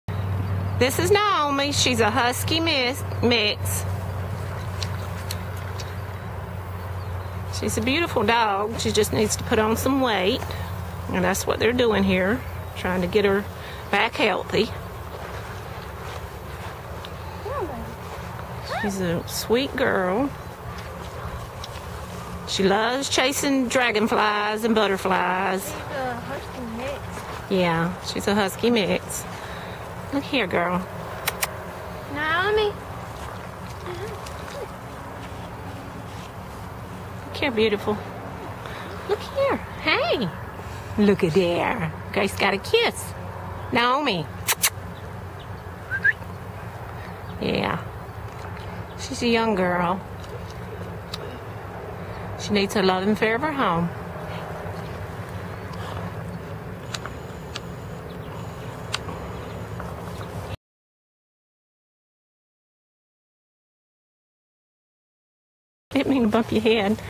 ID#1639, NAOMI, Darlington County Humane Society DSCN43131-Desktop.mov
Darlington County Humane Society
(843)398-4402
ID#1639, NAOMI, female, 2-3 years old, Husky Mix, Intake Date: 5-25-12
Beautiful NAOMI is dreaming and waiting on her Loving Forever Home, who has wide-open spaces for her to run and chase all the butterflies and dragonflies she sees. She is a very adventurous, sweet girly, who is ready to go exploring with you!!
No cats please, because she loves to chase them too!!
Dialogue credited to Susan blakeney:
Just as pretty on the inside as on the outside!  No joke!  Folks might think a pretty, petite husky such as myself might be a bit full of herself, but truth be known, I am totally down to earth, low key and ready to sit and dote on you.    A gentle kisser and very easy leash walker,  I am terrific around other dogs and if they wish to body surf with me,  that's just fine in my book.  I'm a couple of years old,  but folks will tell you I have this wonderful puppy innocence about me.  I guess it means everything is a fun, new experience and I embrace it with awe and wonder.  I'm alert and smart as a whip,  so I think it's safe to say I will bring beauty and brains to my new home!   2-3 yrs old; 5/25/12